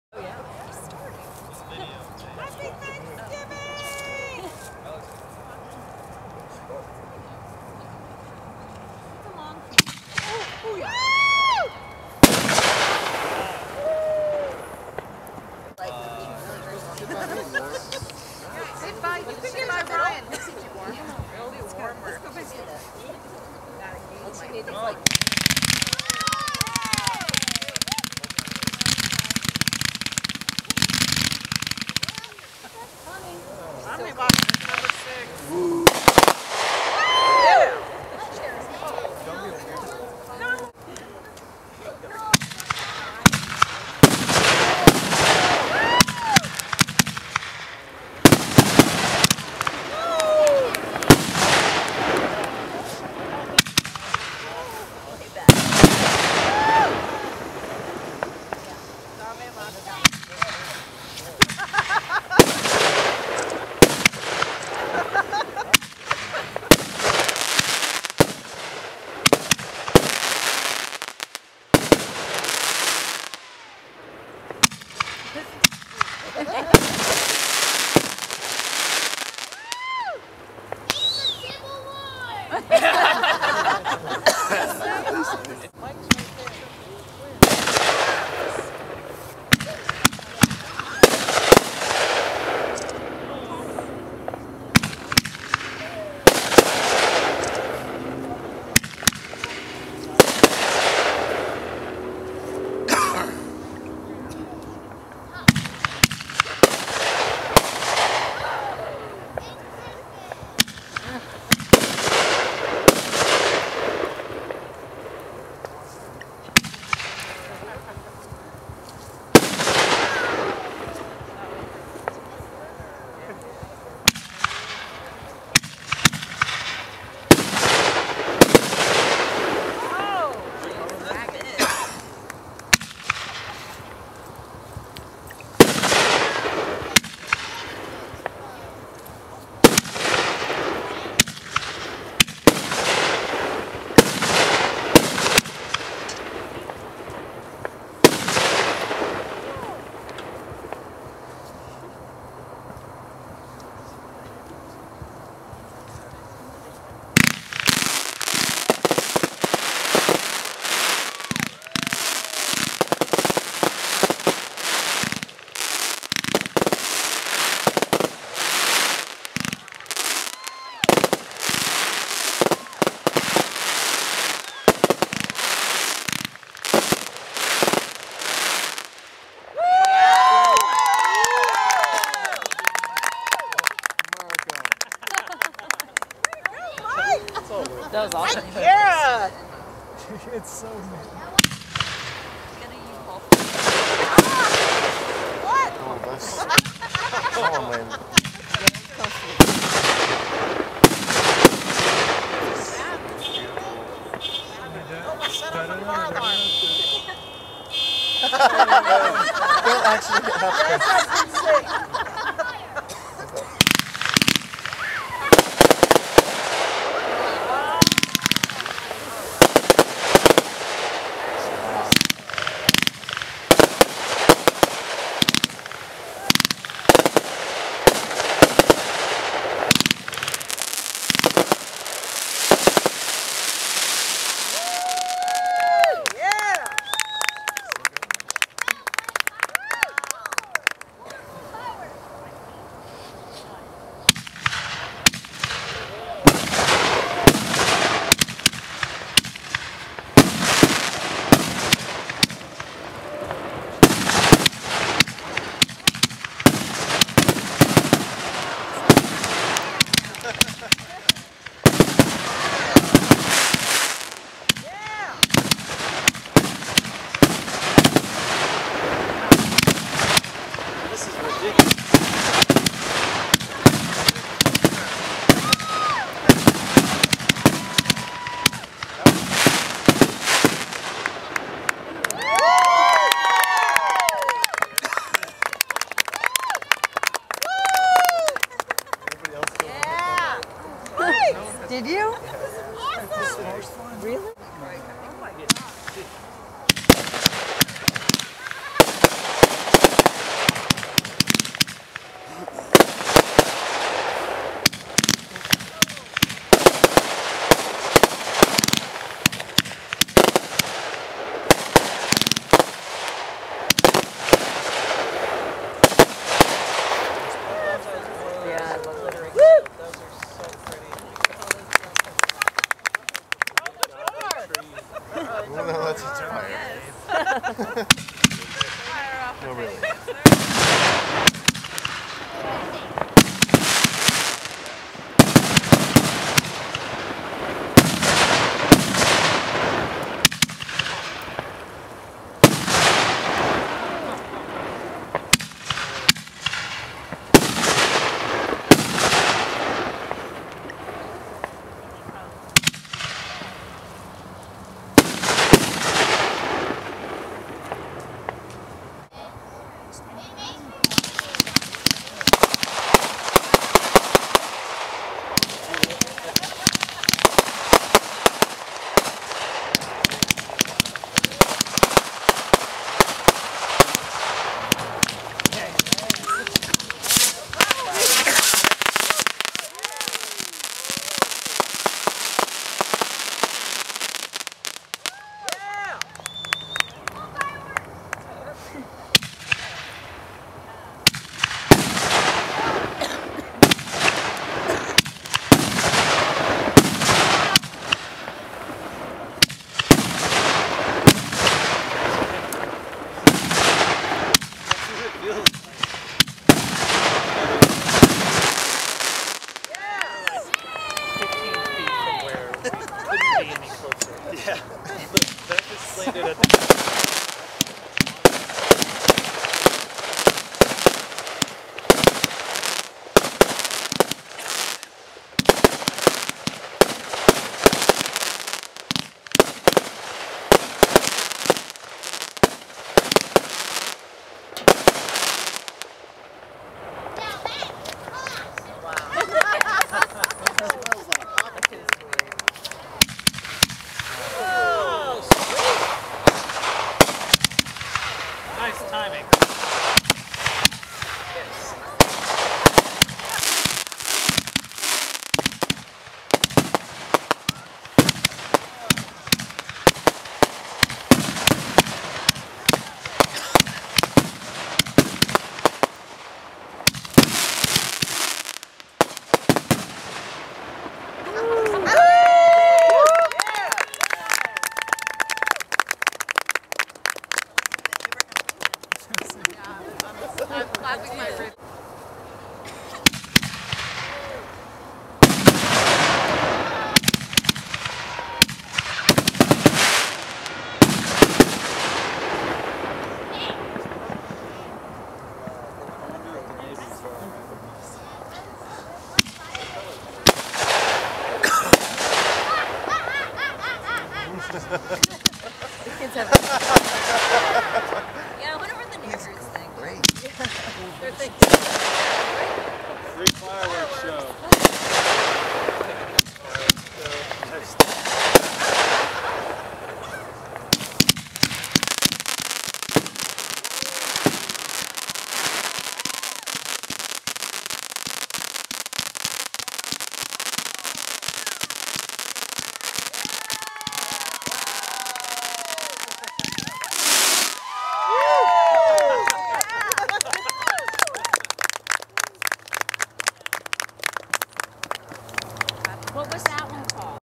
Thanksgiving Fireworks '13
My first fireworks video attempt was less than perfect. I focused more on the show than the video, and cut the video off half way through the show. It just gives a rough idea of how it went.